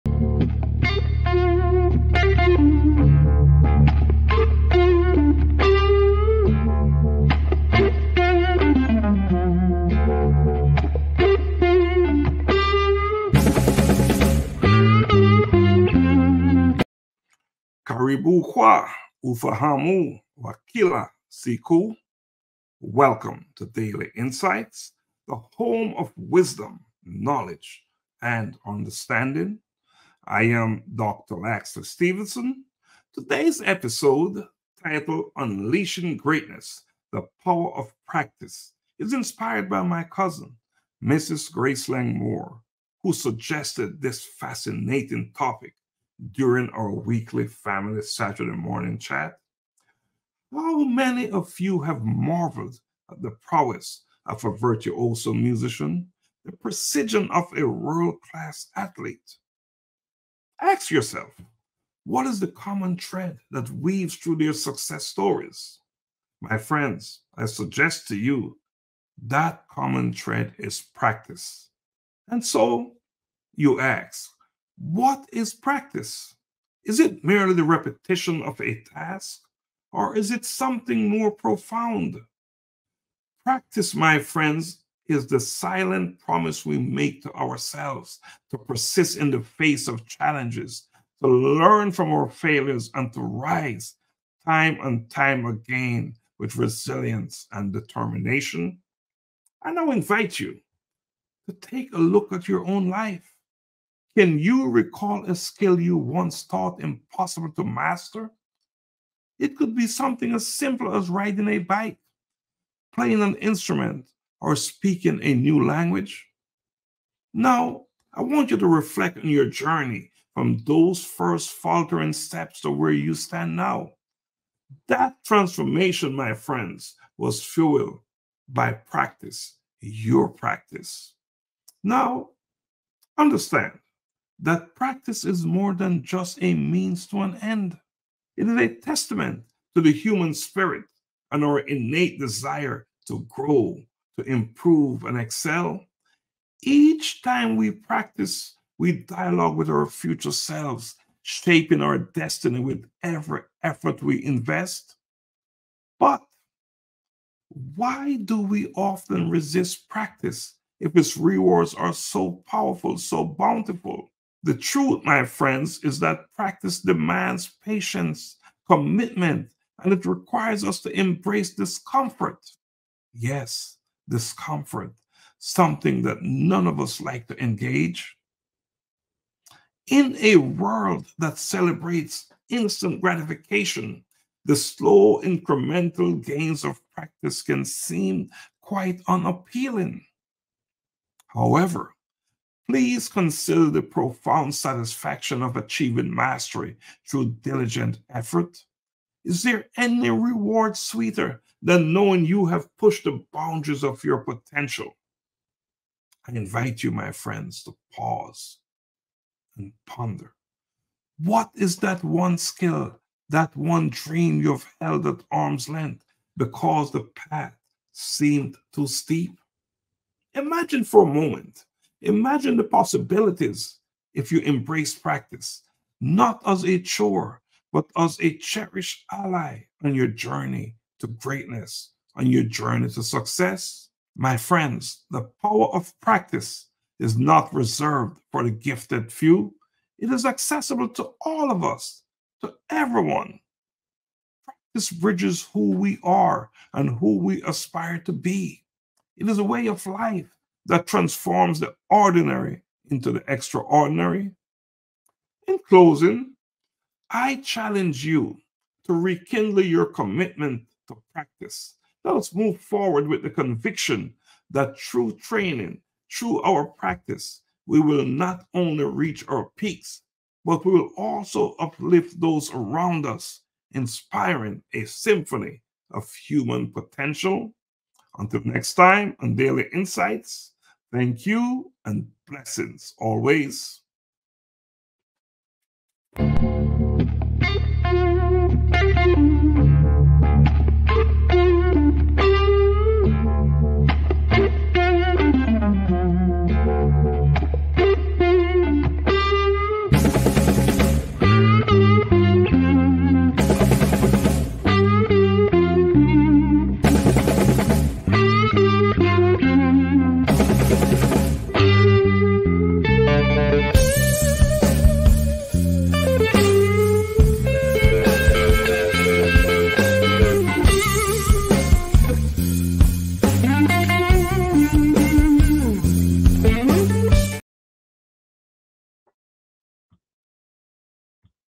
EP56. Unleashing Greatness: The Power of Practice. Daily Insights With Dr. Laxley W. Stephenson
Episode 56. Unleashing Greatness: The Power of Practice. Daily Insights With Dr. Laxley W. Stephenson.

 How many of you have marveled at the prowess of a virtuoso musician, the precision of a world-class athlete, or the skill of a master artisan? Now, ask yourself, what is the common thread that weaves through their success stories? That common thread is practice.
But what is practice, truly? Is it merely the repetition of a task, or is it something more profound? Practice is the silent promise we make to ourselves to persist in the face of challenges, to learn from our failures, and to rise, time and time again, with resilience and determination.

Soft Cover:
A Collision With Purpose: In Pursuit of Purpose

UnEarthing YOU: Your Guide to Self-Discovery and Personal Development